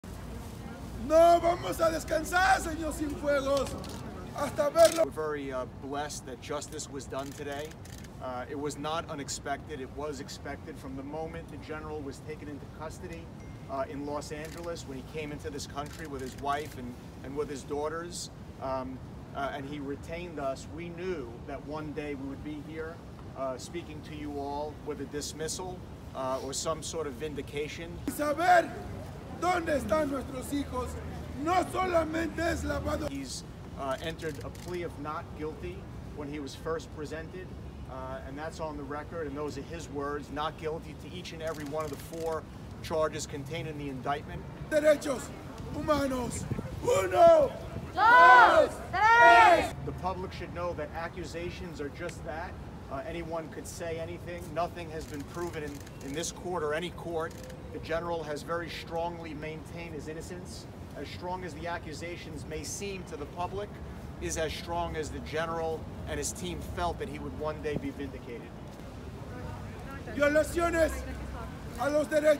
US drops drug charges against ex-Mexican general
U.S. prosecutors on Wednesday formally dropped a drug trafficking and money laundering case against a former Mexican defense secretary, a decision that came after Mexico threatened to cut off cooperation with U.S. authorities unless the general was sent home.
A judge in New York City approved the dismissal of charges, capping a lightning-fast turnaround in the case of former Gen. Salvador Cienfuegos, who was arrested just weeks ago in Los Angeles, but will be returned to Mexico under an unusual diplomatic deal between the two countries.
The decision to drop the case was an embarrassment for the United States, which had touted the arrest as a major breakthrough when Cienfuegos was taken into custody Oct. 15.
But the arrest drew a loud protest from top officials in Mexico and threatened to damage the delicate relationship that enables investigators in both countries to pursue drug kingpins together.
“The United States determined that the broader interest in maintaining that relationship in a cooperative way outweighed the department’s interest and the public’s interest in pursuing this particular case,” Seth DuCharme, the acting U.S. Attorney in Brooklyn, told the judge at a hearing.
He said the decision to drop the charges was made by Attorney General William Barr.
Cienfuegos was secretly indicted by a federal grand jury in New York in 2019.
He was accused of conspiring with the H-2 cartel in Mexico to smuggle thousands of kilos of cocaine, heroin, methamphetamine and marijuana while he was defense secretary from 2012 to 2018.
Prosecutors said intercepted messages showed that Cienfuegos accepted bribes in exchange for ensuring the military did not take action against the cartel and that operations were initiated against its rivals.
He was also accused of introducing cartel leaders to other corrupt Mexican officials.
Mexican officials complained that the U.S. failed to share evidence against Cienfuegos and that his arrest came as a surprise.
It also caused alarm within Mexico's military, which has played a crucial role in operations against drug cartels.
It is rare for a highly prized defendant in a U.S. case to be arrested and then released in short order for reasons of diplomacy.
 But it’s not unprecedented, though historically it has been more likely to occur in cases involving espionage than drug trafficking.
U.S. prosecutors in Manhattan have recently resisted diplomatic efforts by another U.S. ally, Turkey, to get charges dropped against a large state-owned bank accused of violating sanctions on Iran.
Cienfuegos, a general who led Mexico’s army department for six years under then-President Enrique Peña Nieto, was the highest-ranking former Mexican Cabinet official arrested since top security official Genaro Garcia Luna was arrested in Texas in 2019.
Analysts said Cienfuegos is unlikely to face charges in Mexico.
Outside the Brooklyn courthouse, defense attorney Edward Sapone said he expected Cienfuegos to be returned to Mexico Wednesday.
He noted that Cienfuegos has pleaded not guilty and had planned to prove his innocence.
“We're very blessed justice was done today,” Sapone said as people protesting his client's release carried signs and chanted nearby.
He added that his client’s plans “in the immediate future are to remain with his family as a free man.”